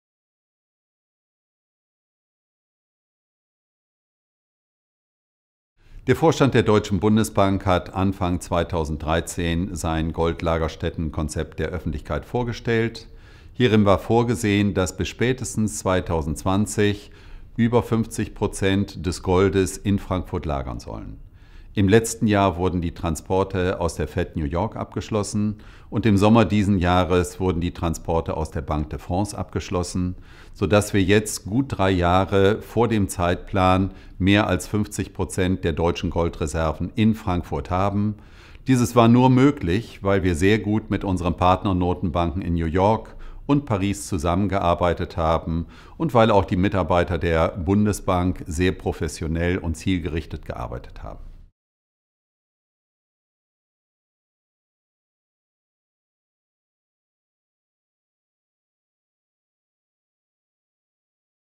La Bundesbank achève le transfert de ses réserves d’or plus tôt que prévu
La Deutsche Bundesbank a achevé trois ans plus tôt que prévu le transfert de ses réserves d’or de New York et de Paris à Francfort-sur-le-Main. L’objectif du concept des sites de stockage formulé en 2013 a donc été atteint avant la date prévue. Un peu plus de la moitié des réserves d’or allemandes se trouve désormais en Allemagne.

Besuchen Sie uns auch auf anderen Plattformen und schreiben uns bei Fragen:

Adresse:
Deutsche Bundesbank

Wilhelm-Epstein-Straße 14
60431 Frankfurt am Main